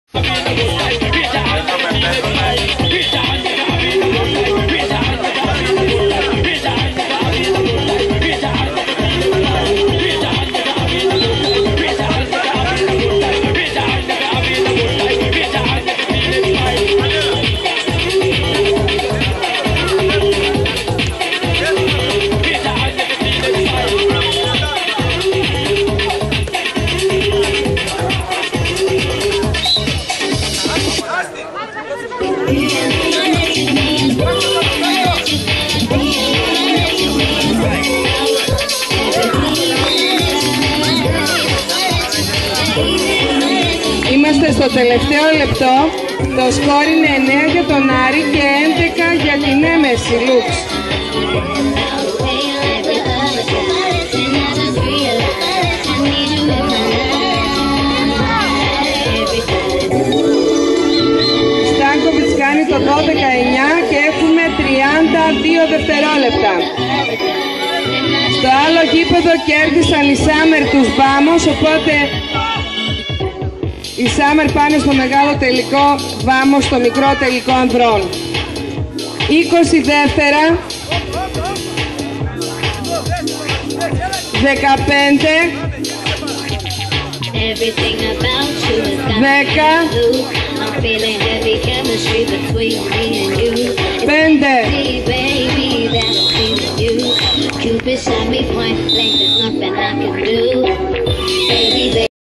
Meliti Cup 2008 Beach Handball - Corfu Greece
Meliti Cup 2008 Beach Handball 9o Πανελλήνιο Πρωτάθλημα χειροσφαίρισης (Μπιτς Χάντμπολ)
Παραλία Ισσος Δήμου Μελιτειέων.